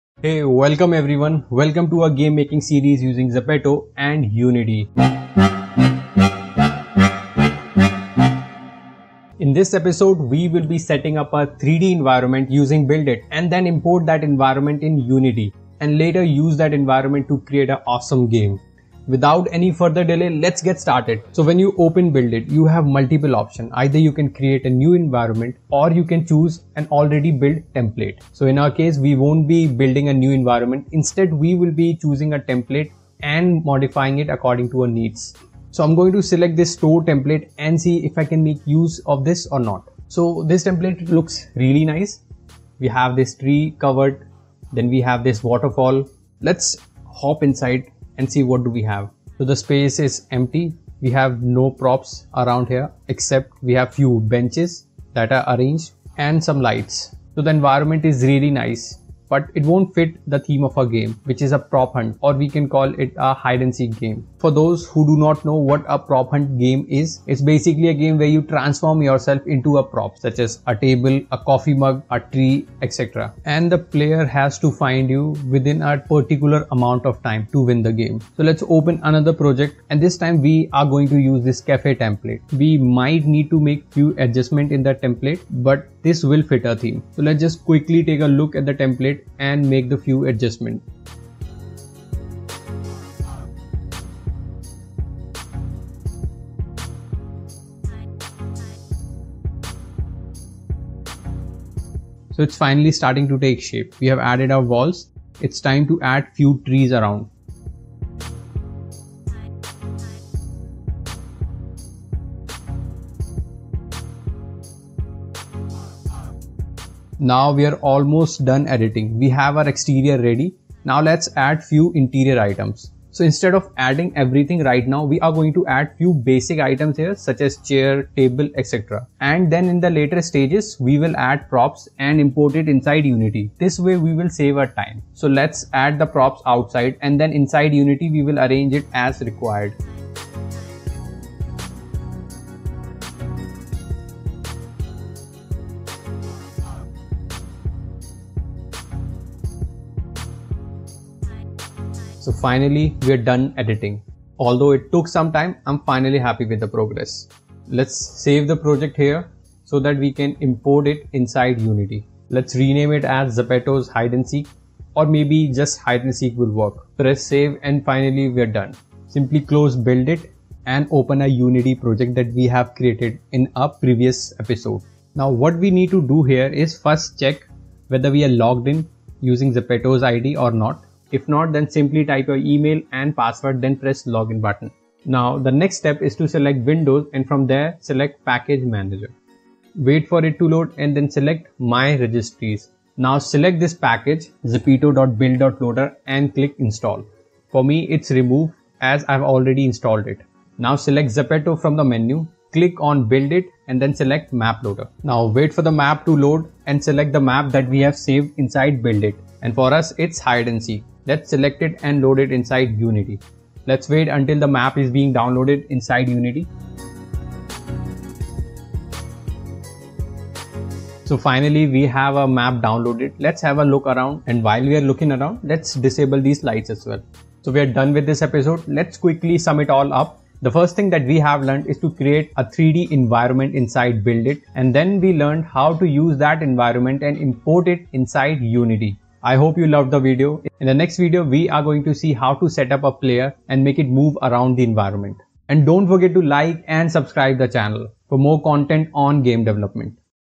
Mastering Build-It: Designing Your Own 3D World || Zepeto and Unity's Video Series - 03
Join me on an exciting journey as I set up Build-It and create an incredible 3D environment from scratch! Watch as I use my creativity to design and build a stunning world using the powerful tools available in Build-It. With every step, I'll show you how to bring your ideas to life and take your 3D design skills to the next level.

Once the environment is complete, I'll show you how to import it directly into Unity, where we'll further develop and enhance it. With Unity's incredible capabilities, we'll be able to add dynamic lighting, shadows, and other effects that will make our world come alive.

Whether you're an aspiring game developer, a 3D artist, or just a curious viewer, this video is for you. Join me as we explore the exciting world of 3D design and learn how to create immersive environments that will captivate your audience.

BuildIt Unity 3DEnvironment GameDevelopment 3DArt DynamicLighting Shadows ImmersiveExperience 3DDesign VideoTutorial GameDesign.